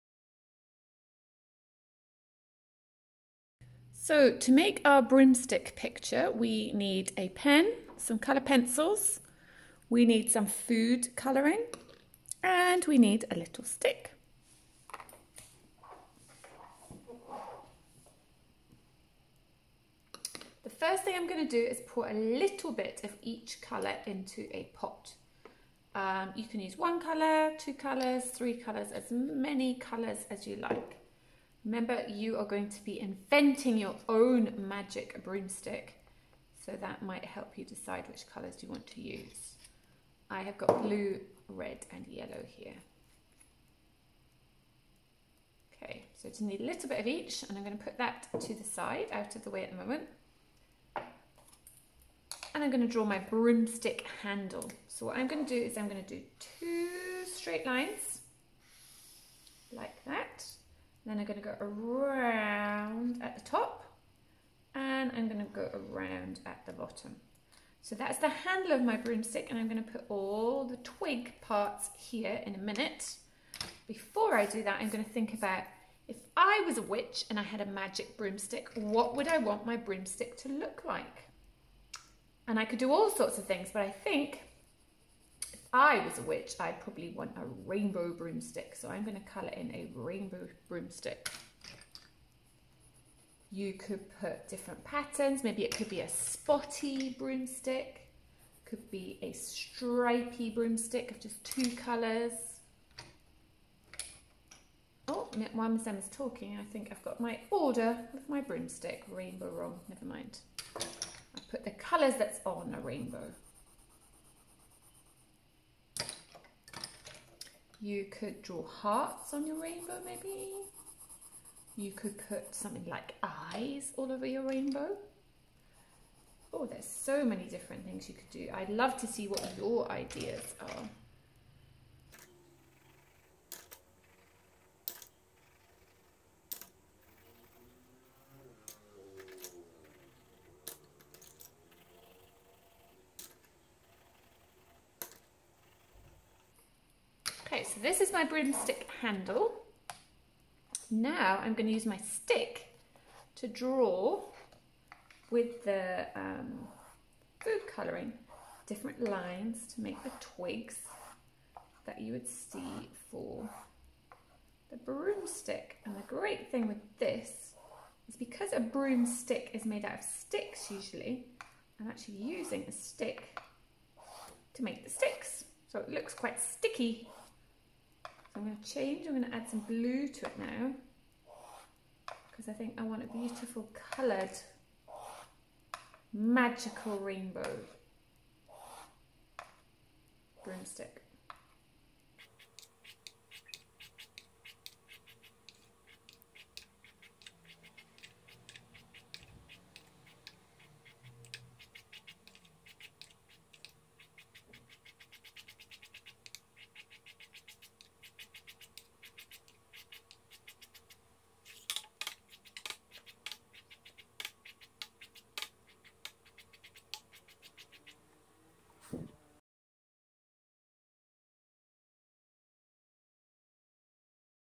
Making a Magical Broomstick
Taking inspiration from Room on the Broom by Julia Donaldson use your imagination and a few simple materials to make this magical broomstick. What magic things might your broomstick be able to do?